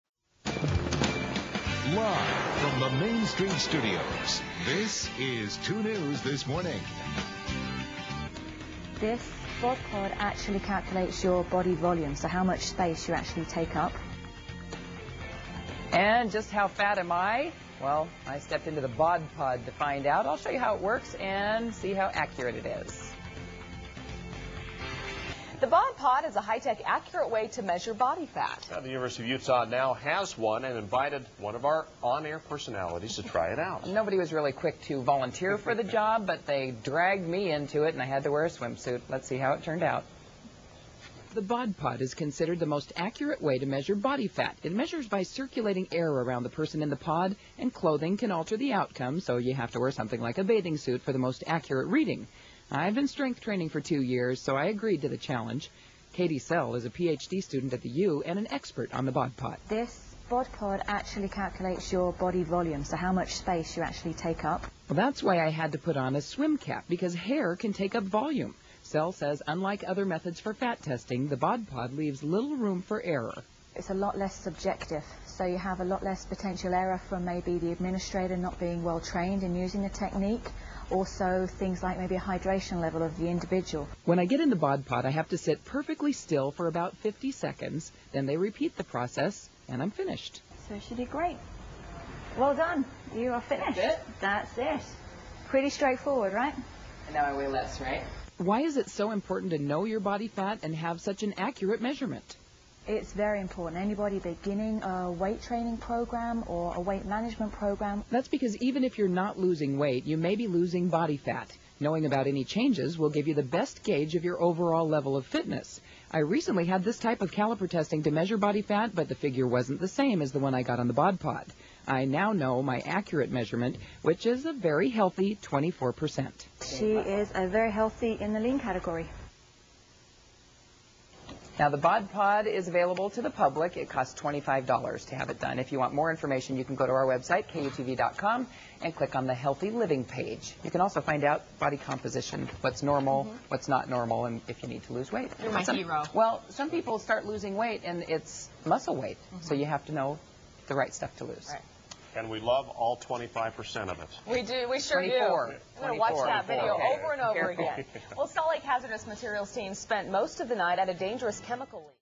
High Tech BOD POD Measures Body Fat
University of Utah uses the BOD POD to analyze body composition for research in health and fitness.